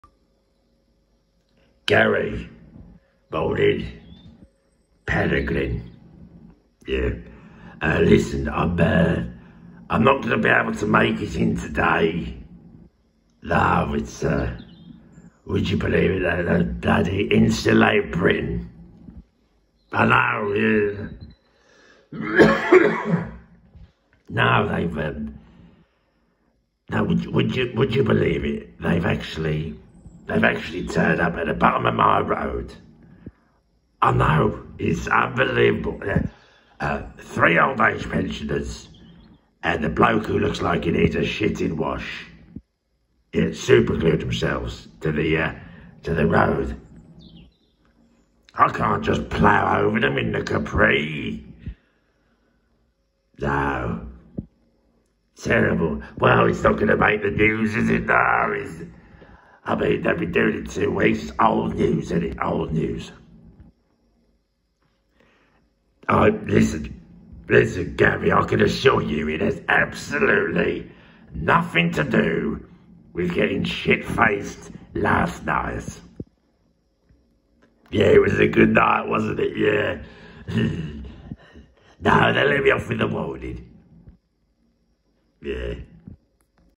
I Want a day off.
I want a day off because I got shitfaced last night.

Just blame the shitting protesters.